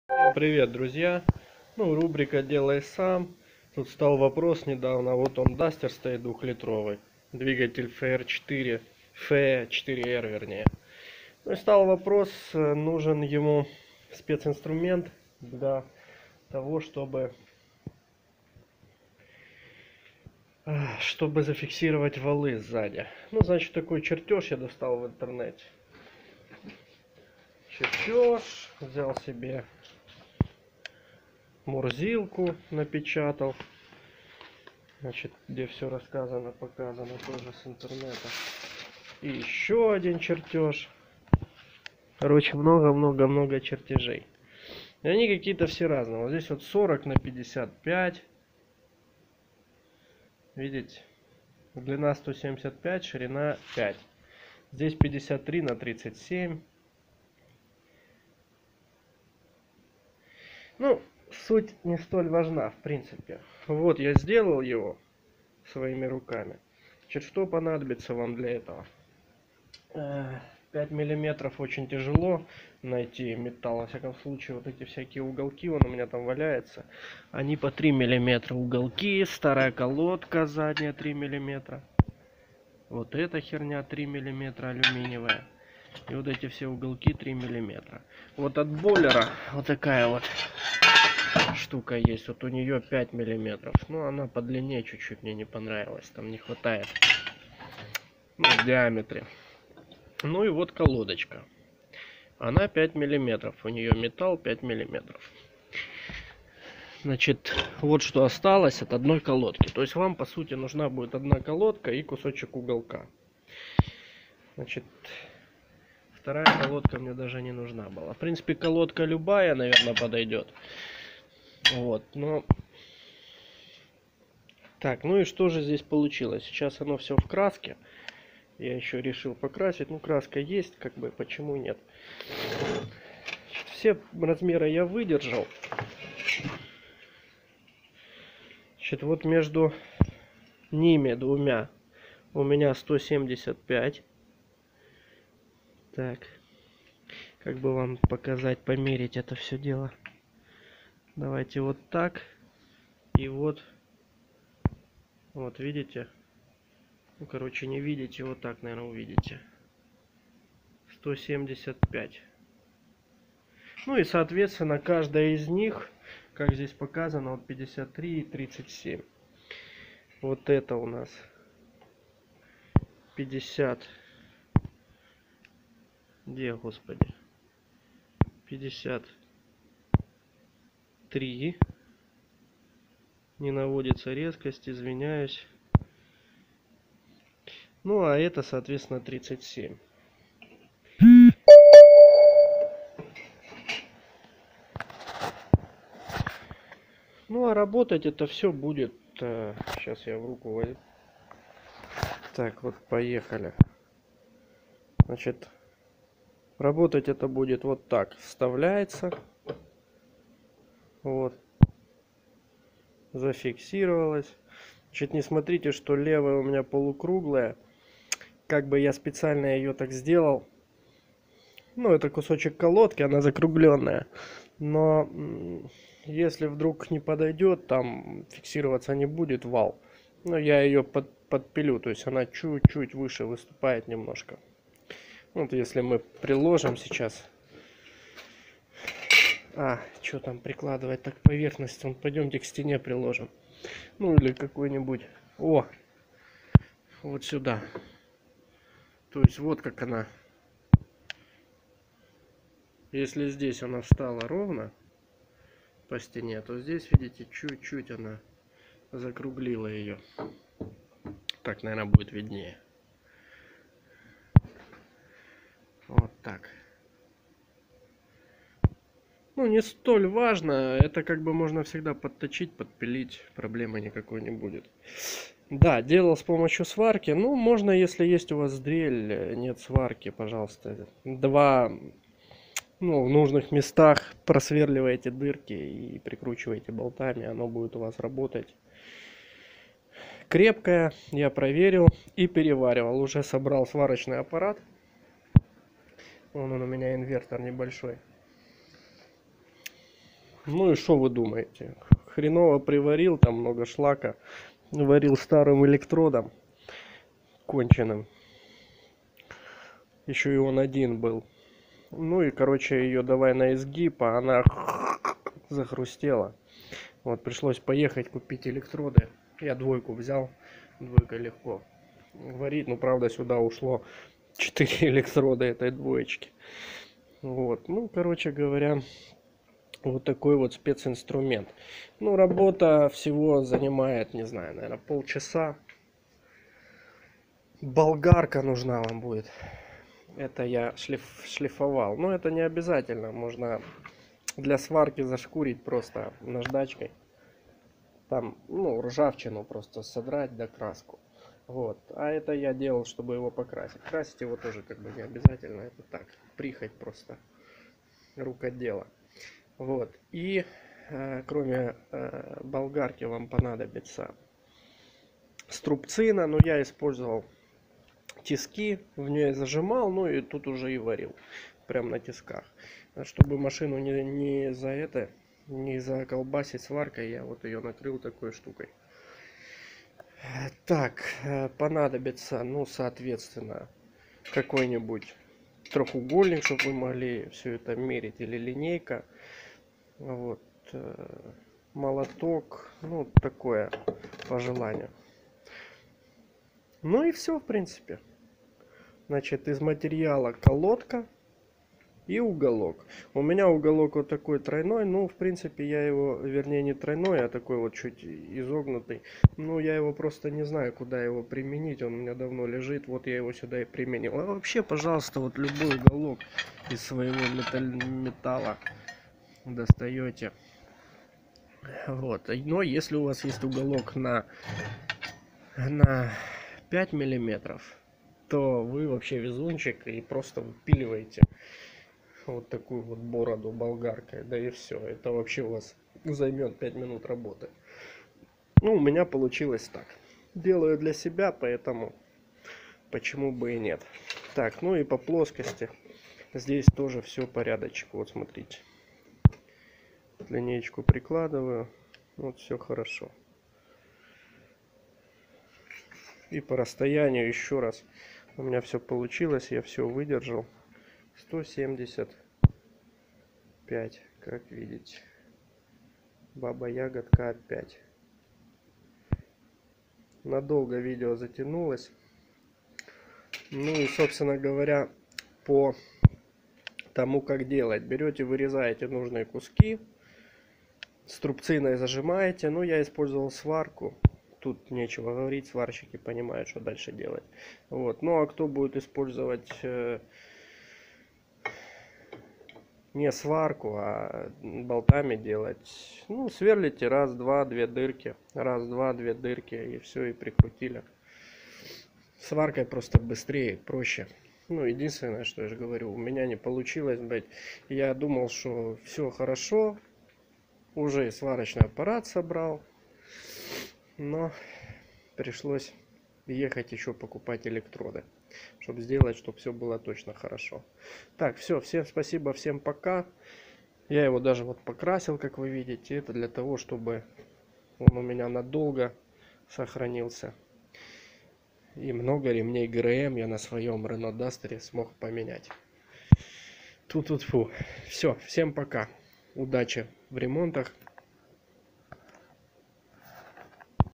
Кронштейн для фиксации распредвалов на Renault своими руками
Изготовление спец-инструмента для фиксации распредвалов на Renault/Nissan

Автор старается!
Поддержи бедолагу подпиской на канал!